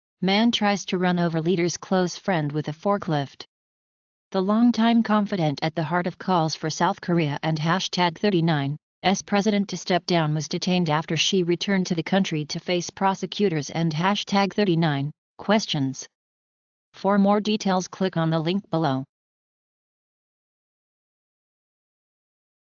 Man tries to run over leader's close friend with a forklift | By : CNN
The longtime confidante at the heart of calls for South Korea&#39;s President to step down was detained after she returned to the country to face prosecutors&#39; questions.